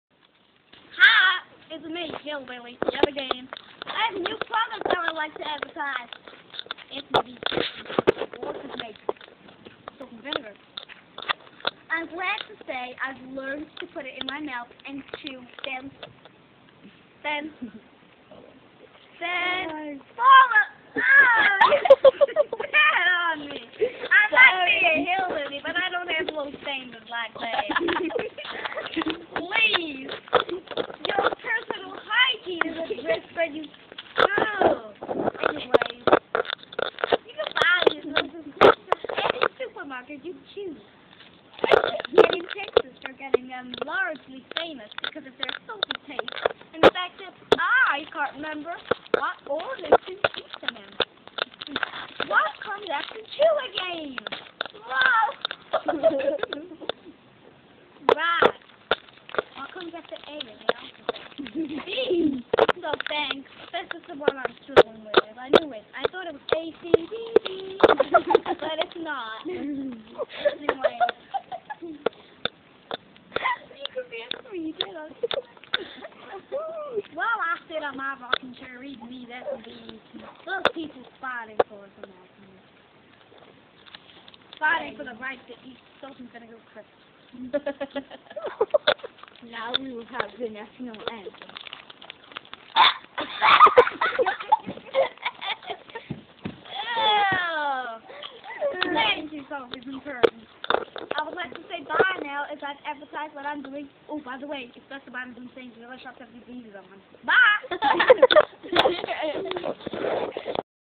Jess advertising S&V's
Jess advertising Salt&Vinigar crisps. (once again with her american accent)